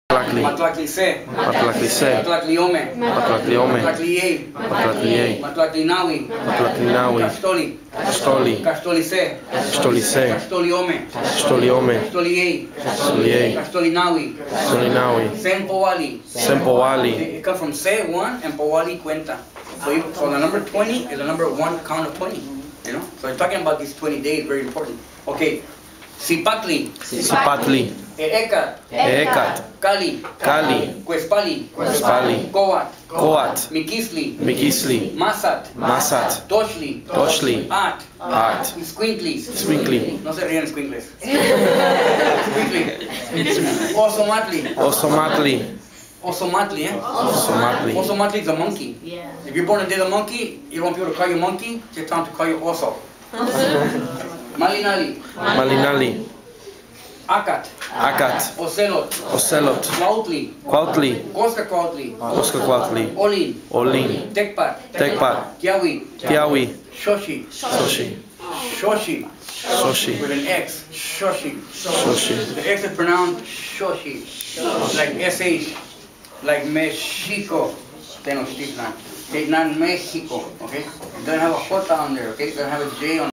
Learning about our math, numerology, astrology, architecture with Mazatzin
ARISE High students from Adelante/Homies Empowerment traveled to San Jose State to learn math, astrology, architecture, agriculture, cosmology and so much more from the elders including Mazatzin.